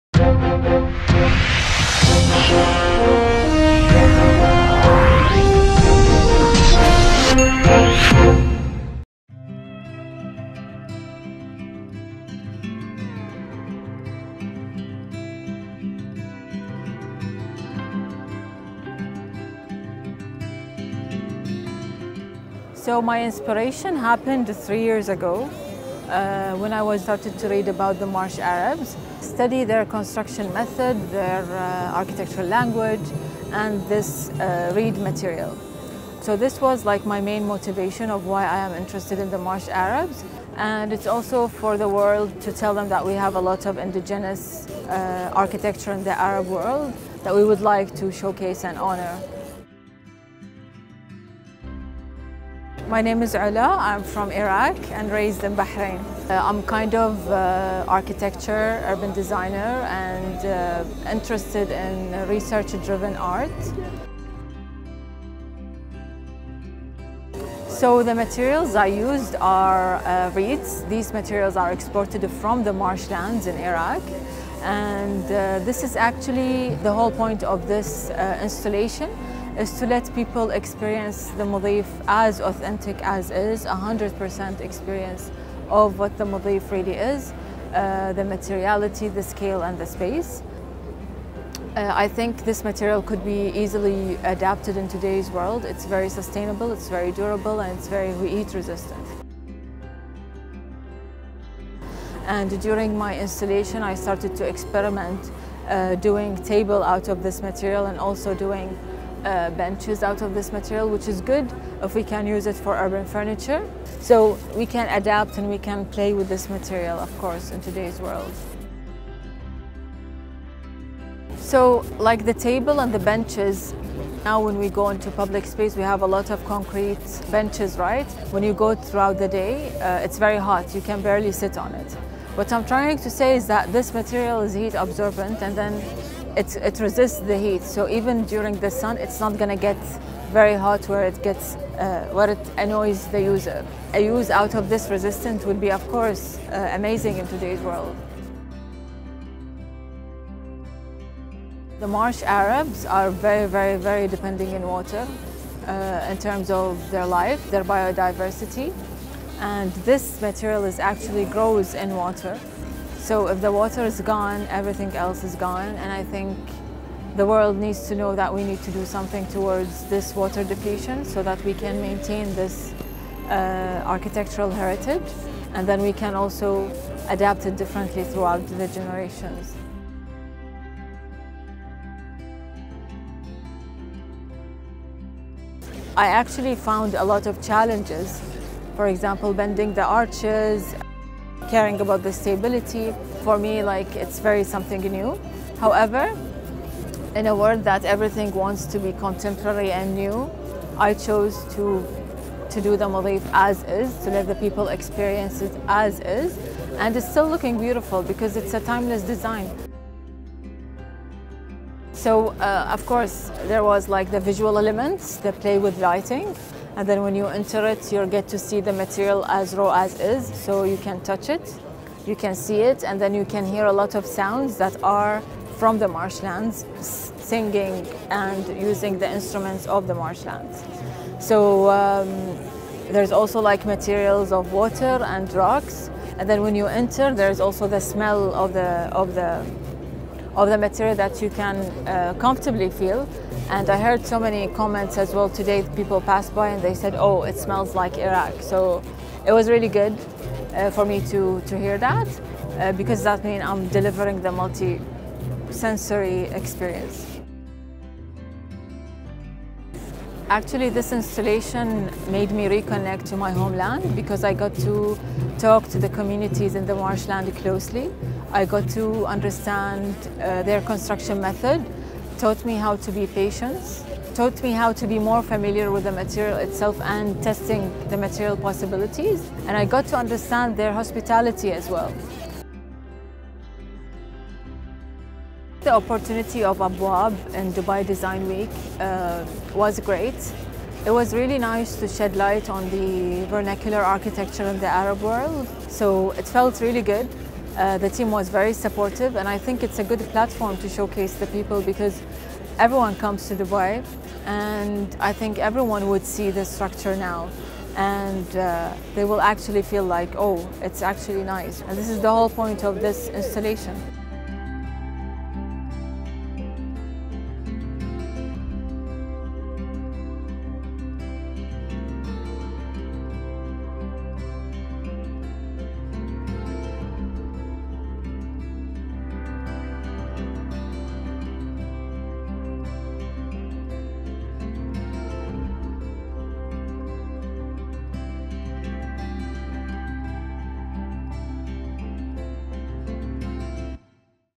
A Present/Absent Mudhif by Ola Saad Znad
Dubai Design Week 2024: Opening Doors to Tradition, Innovation, and Regeneration -  A Present/Absent Mudhif by Ola Saad Znad
Constructed from locally sourced reeds and mud, this piece reflects the traditional structures used by Iraq's Marsh Arabs, which have sustained these communities for generations. Iraqi designer Ola Saad Znad pays homage to this heritage with an installation that symbolizes the severe impact of water depletion on the once-vibrant marshlands. The fragmented design features multi-sensory elements, reimagining the Mudhif as an open, inclusive space that emphasizes sustainable materials and the deep cultural significance of traditional techniques, making them relevant in contemporary contexts. By Soniya Kirpalani &  Visuals By Santosh Rai